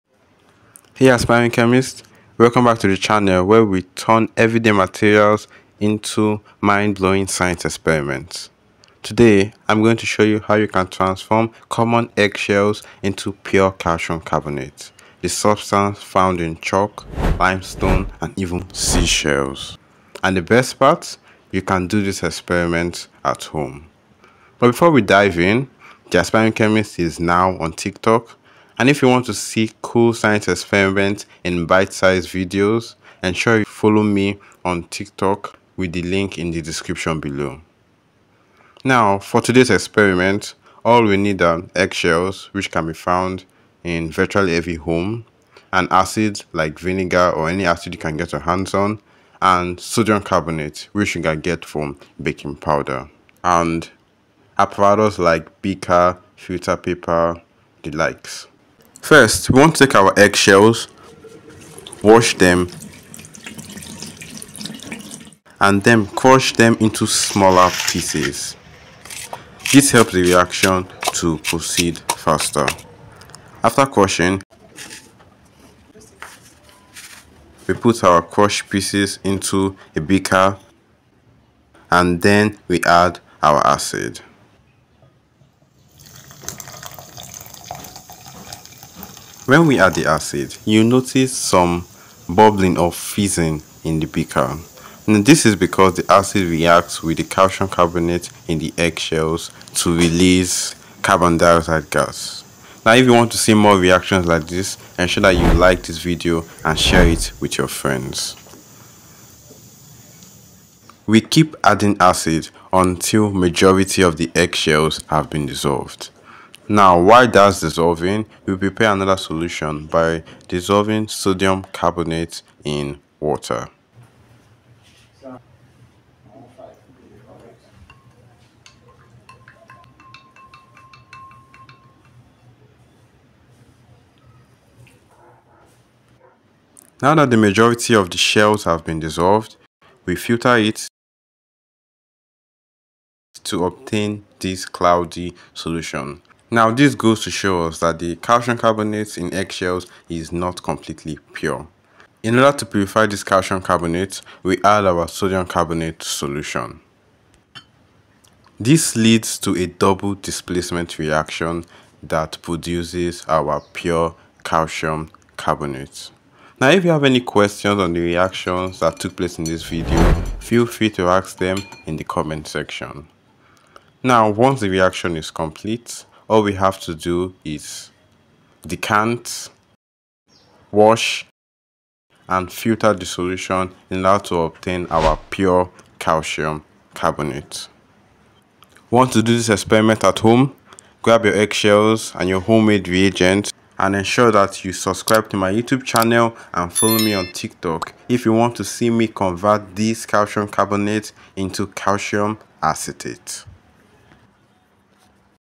How To Make Calcium Carbonate (CaCO3) At Home
Ever wondered how to make Calcium Carbonate (CaCO₃) at home using simple materials?  In this video, I’ll show you step-by-step how to extract CaCO₃ from everyday eggshells using Hydrochloric Acid (HCl), making a pure compound with numerous uses

Calcium Carbonate is everywhere — from chalk to seashells to limestone. This DIY chemistry experiment is not only fun but also educational, helping you understand one of the most important compounds in nature. Perfect for students, science enthusiasts, or anyone who loves hands-on experiments. ✨

0:00 Introduction
0:40 Materials
1:00 Procedure
1:24 Adding HCl
2:20 Filtration
2:36 Adding the Na2CO3
2:57 Purification of CaCO3
3:09 Conclusion